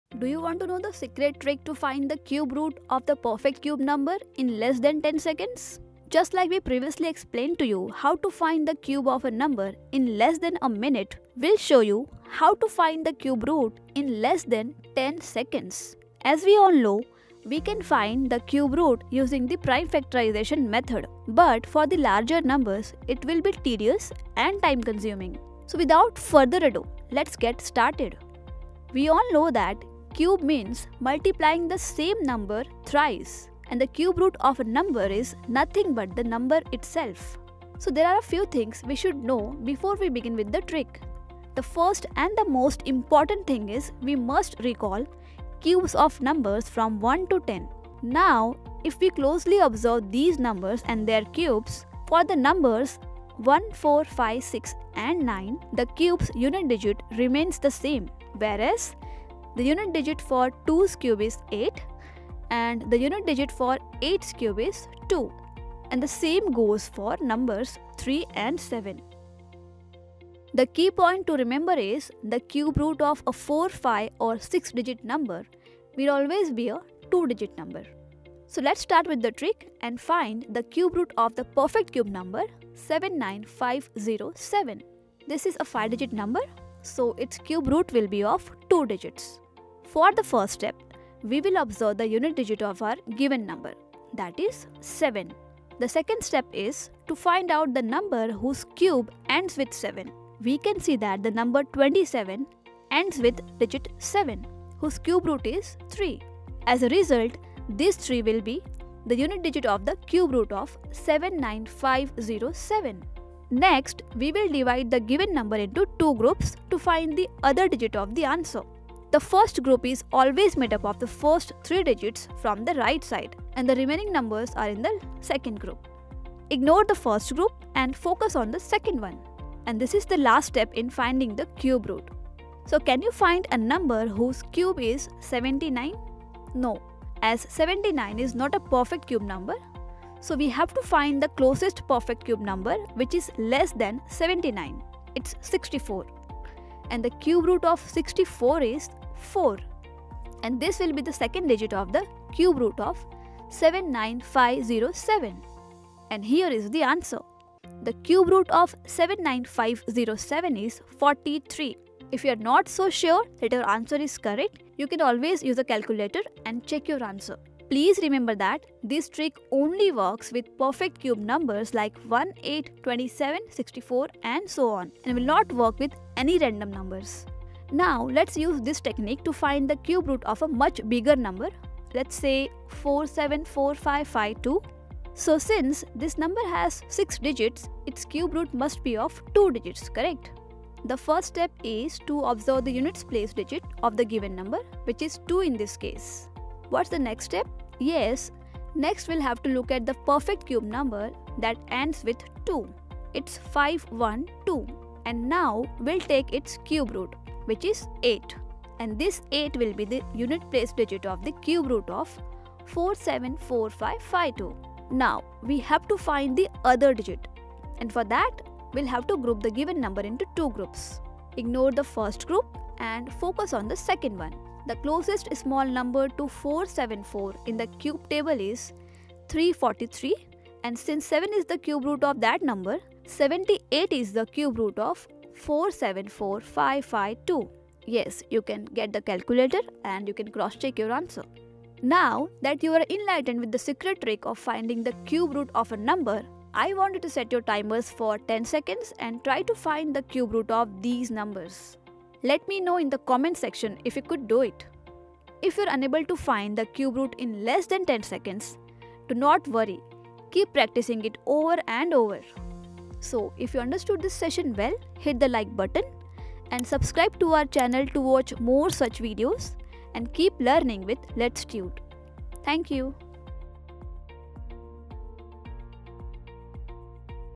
How To Find Cube Root of Perfect Cube Number In Just 10 Sec? | Best Cube Root Math Tricks| Letstute
Hello Friends,
Check out the video on "How to Find Cube Root of Perfect Cube Number in Just 10 Seconds?  | Best Cube Root  Tricks for Fast Calculation in Math by Letstute.

In this video, we have found the Cube Root of numbers in just 10 seconds without using a calculator.
A cube root of a number is a value that when we multiplied a number thrice times by itself.

In this Video:
0:00 Introduction
0:30 Meaning of Cube & Cube Root
0:47 Recall the Cube of numbers from 1 to 10
1:11 Key Point to remember
1:21 Example
1:31 Step 1 Observe the unit digit of the given number.
1:31 Step 2 Find the no that ends with the same no in the table & find its cube root
1:54 Step 3 Create two groups i) The Last three digits of given no ii) The remaining no
2:10 Step 4 Find the cube root of the 2nd group no or its nearest small no and this will be 10th          digit of the required answer.
2:49 This trick only works for perfect Cube Numbers.
4:04 Conclusion

Also, check our Top selling complete course on Accountancy on Udemy Course